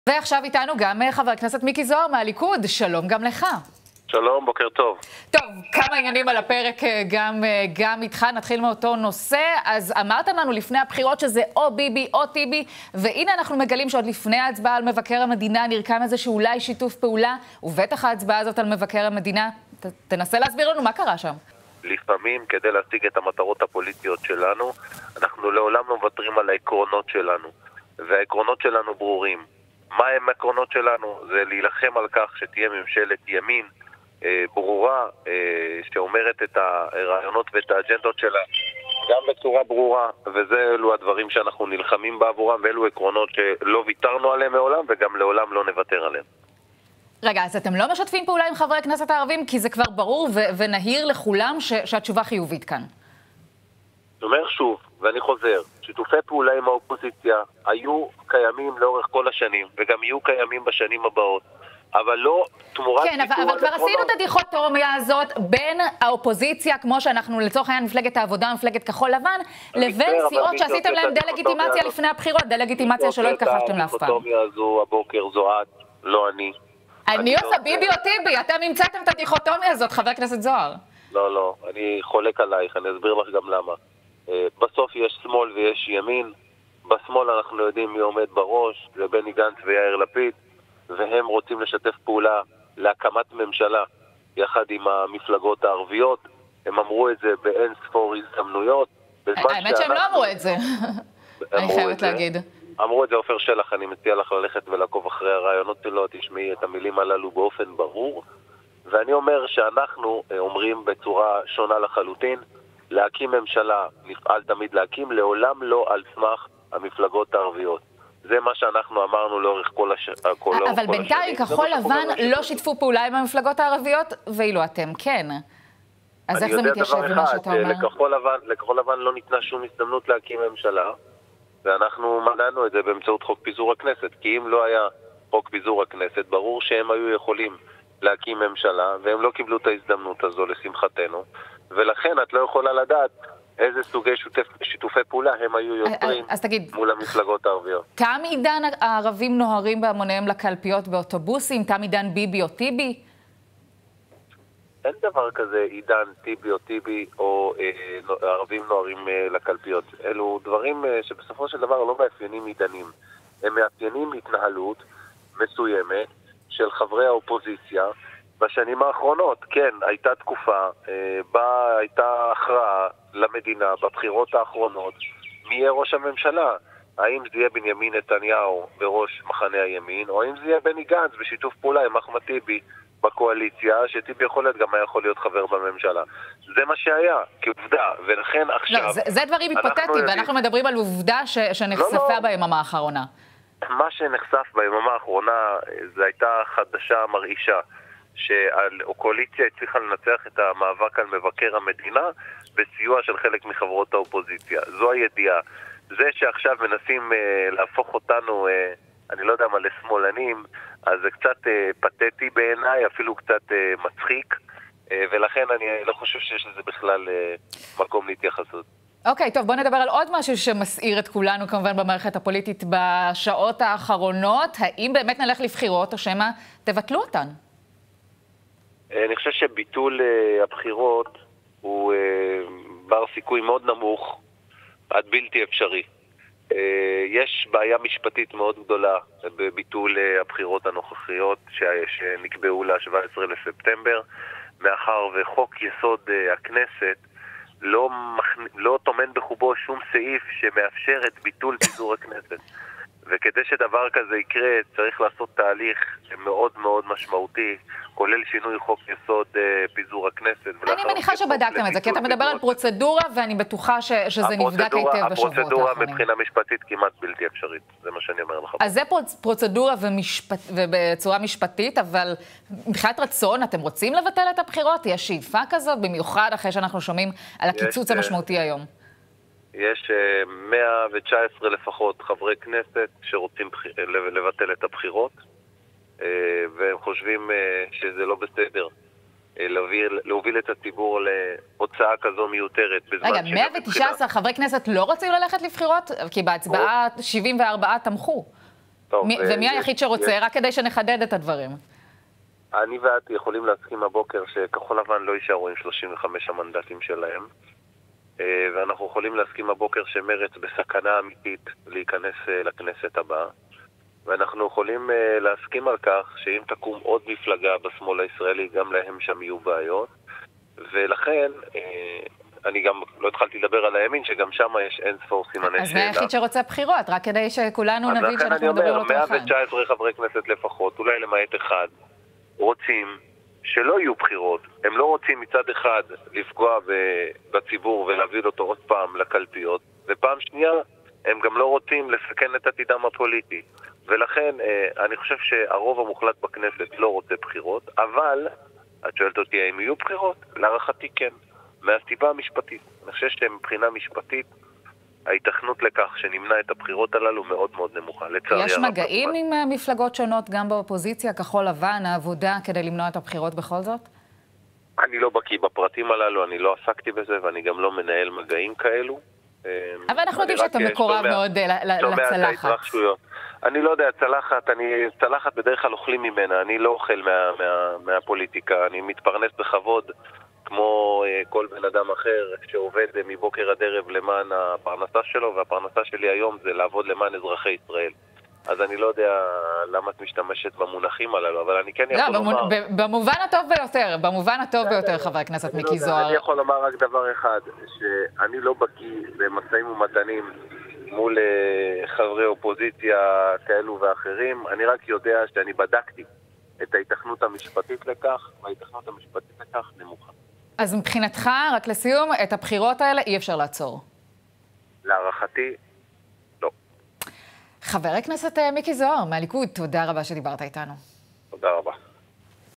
שיתוף הפעולה של הליכוד והח"כים הערבים: ריאיון לאולפן עם ח"כ מיקי זוהר
שיתוף הפעולה של הליכוד והח